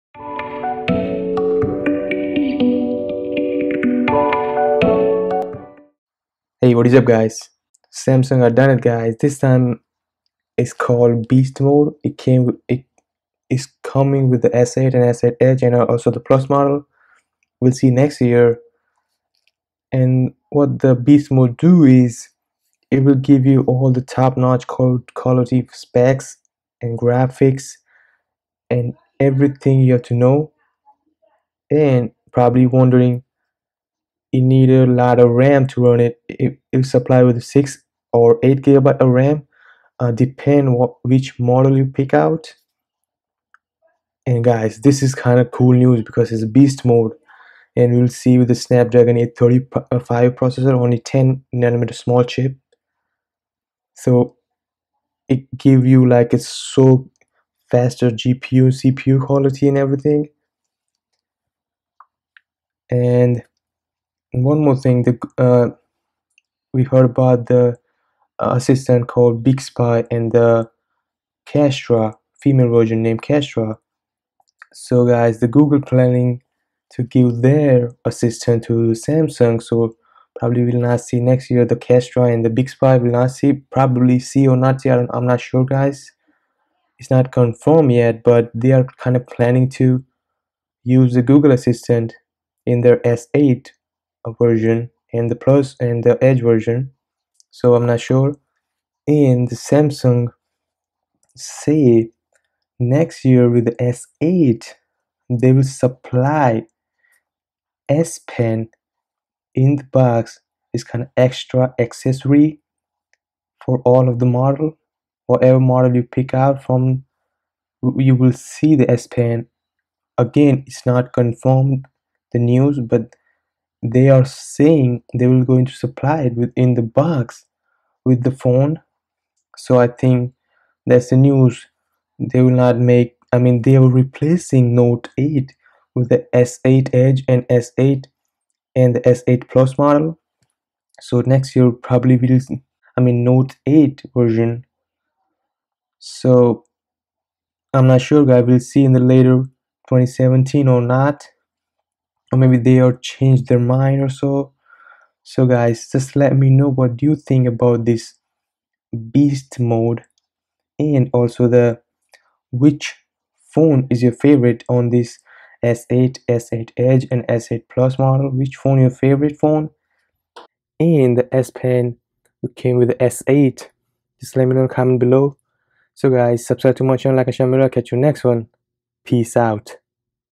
Samsung Galaxy S8 - BEAST MODE !!!
Samsung Galaxy S8 latest news, leaks and reports. BEAST will be unleashed. The GOOGLE Assistant, 8GB RAM will come with galaxy S8 & EDGE, PLUS !

Samsung Galaxy S8, S8 Plus, S8 Edge. The comeback monster to rock a S PEN!